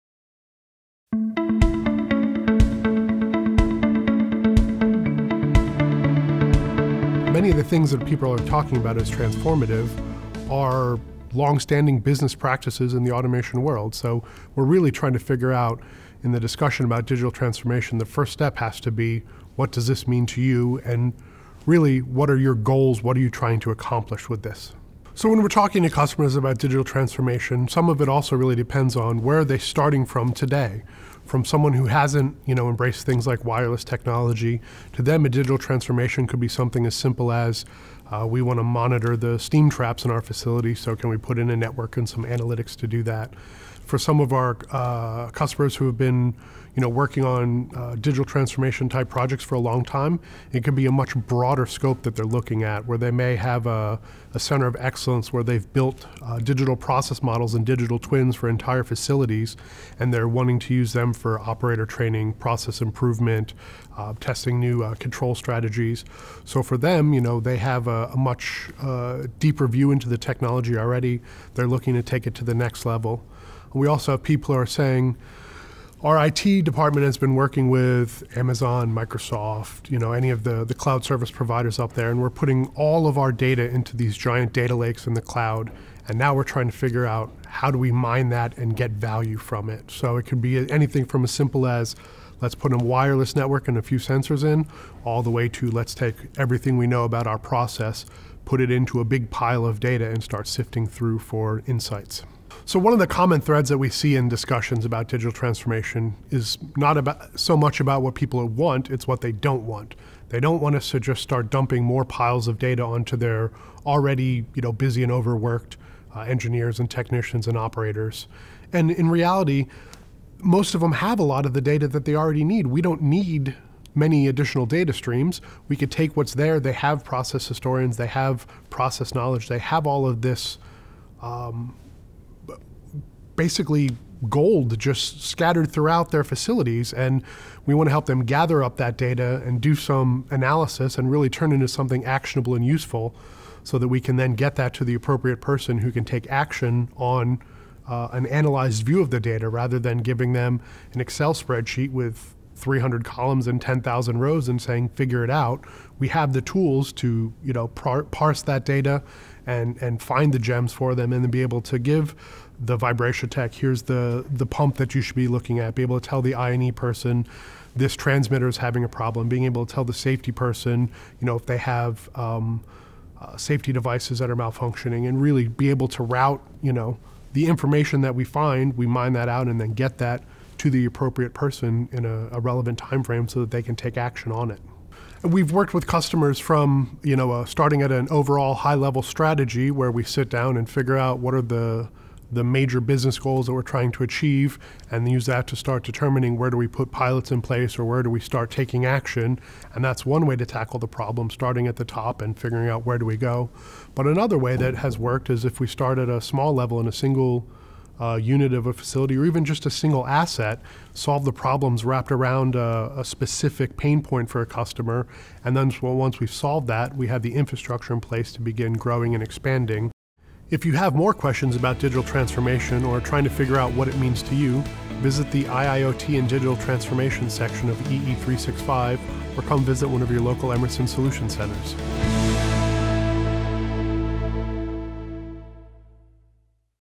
Digital Transformation: Let's Start from the Top (or the Bottom)
For Digital Transformation, identifying your goals & choosing a starting point is a challenge for all organizations. Solutions Architect, Brian Atkinson, breaks down mapping & entry points in this EE365 video. Explore IIoT & Digital Transformation here: